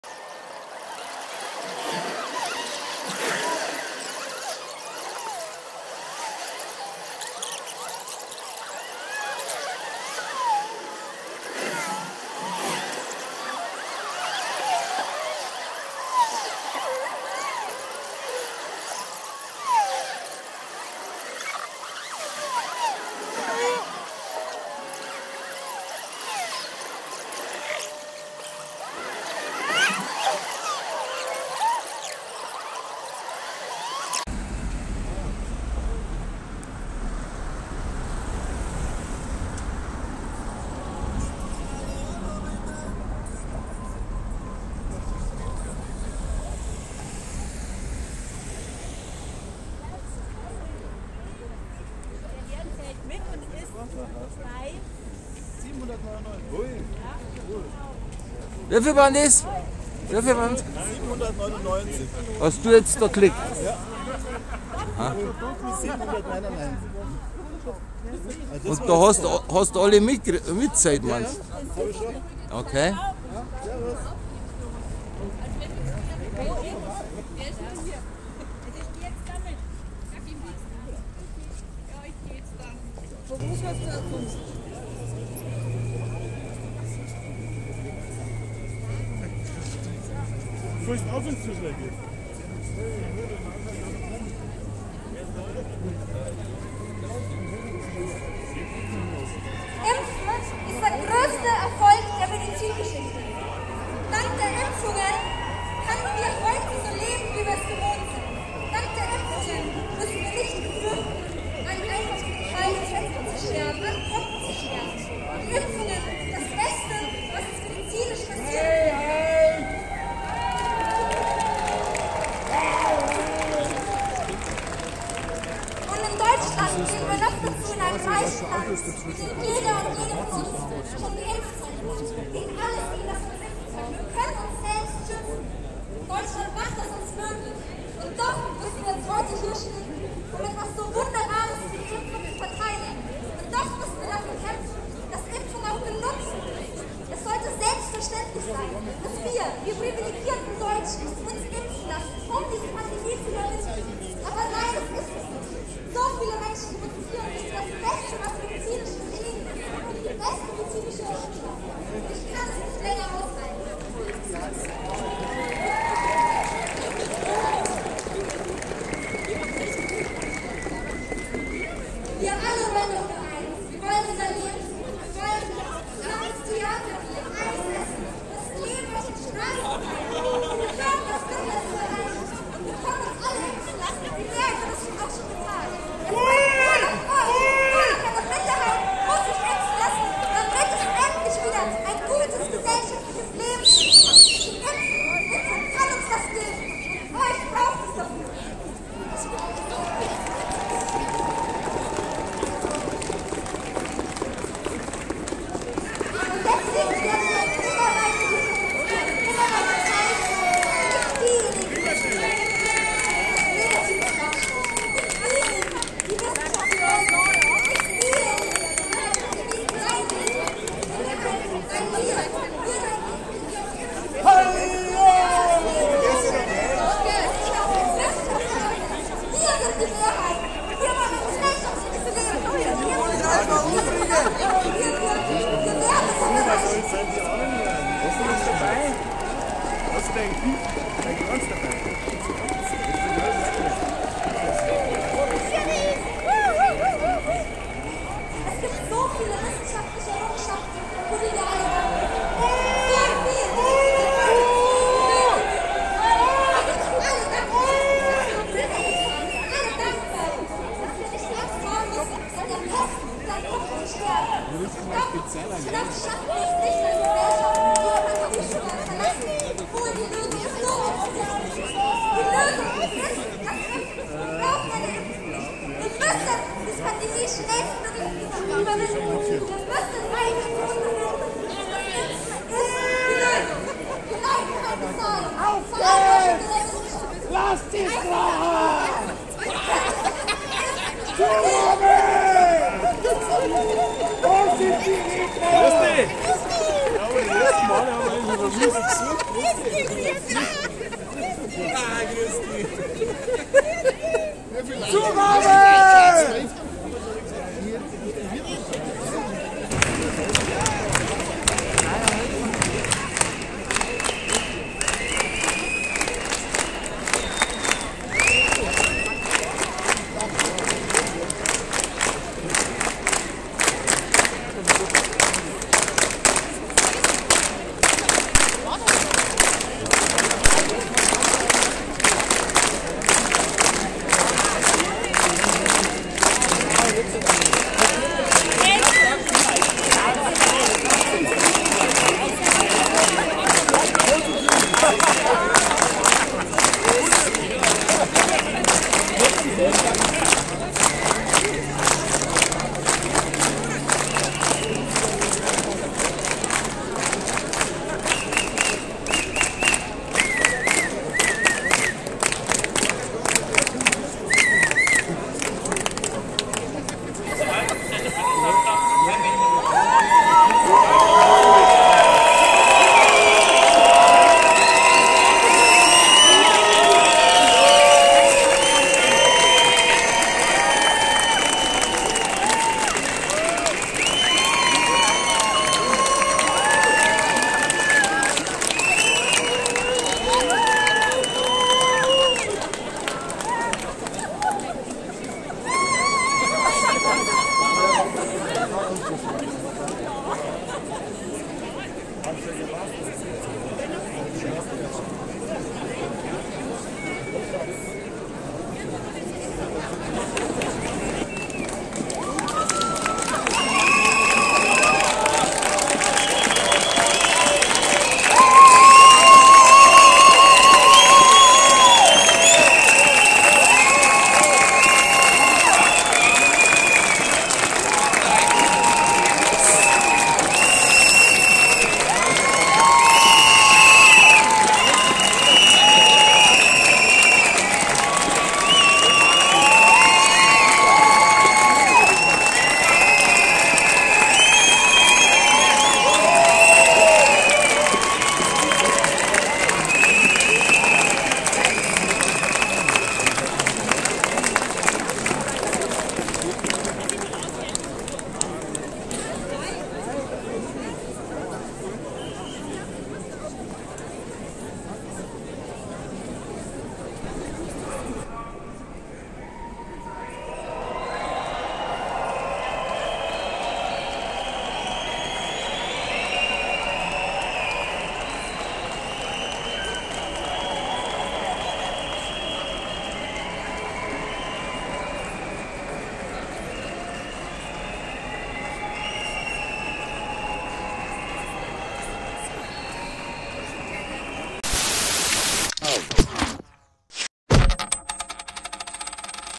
14.02.2022 Landshut | Zusammenschnitt | ca. 4000 🗽🚶🚶‍♂️🏃🏻🏃🏻‍♂️
Valentinstag 2022 - der Tag der Liebe - in Landshut mit Stimmung. Rund 4000 Fußgänger flanieren gegen 18 Uhr verteilt durch Landshut. Die einen gehen an der Isar entlang und die anderen schauen sich mal die Freyung an. Nachdem sich dann alle Flanierer auch das Rathaus ansehen wollten, redete sich im Vernunft-Käfig davor so manche Impffaschist*in in Rage. Die Landshuter Passsanten kommentierten das mit dem seit dem Jahr 1475 traditionellen Stadt-Rufen "Halloo!!" und "Zugabe!!", denn da steigt  bekanntermaßen die Stimmung. Sogar die Polizei hatte leichte Probleme, sich Gehör zu verschaffen, deren Durchsagen wurden schlicht übertönt.